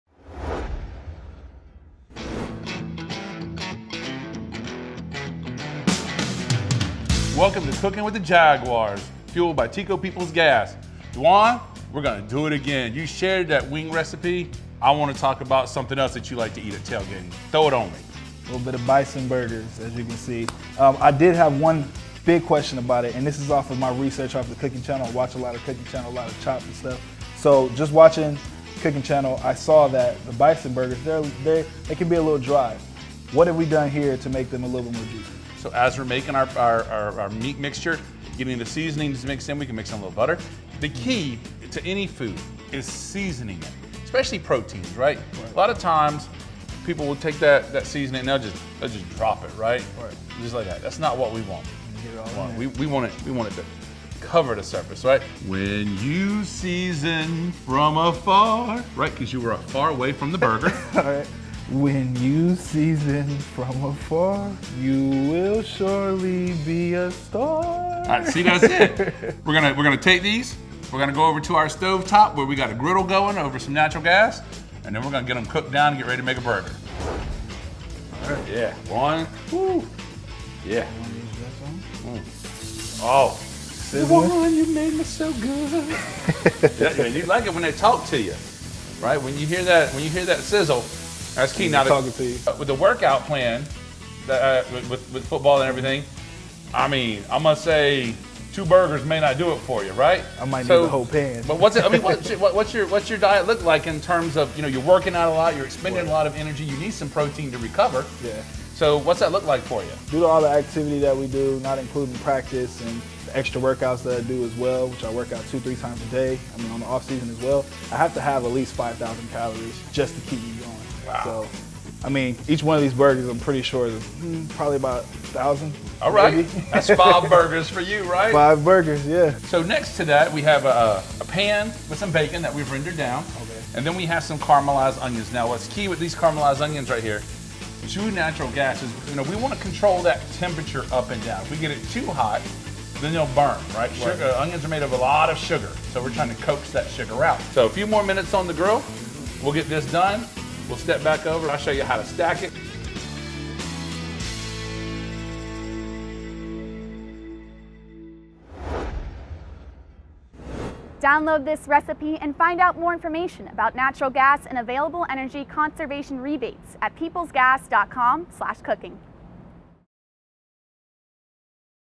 Cooking with the Jaguars: Bison Burgers
Defensive lineman Dawuane Smoot joins Publix's Chef Tony in the kitchen to learn how to make Bison Burgers over a TECO People's Gas natural gas stove.

Full recipe: PeoplesGas.com/cooking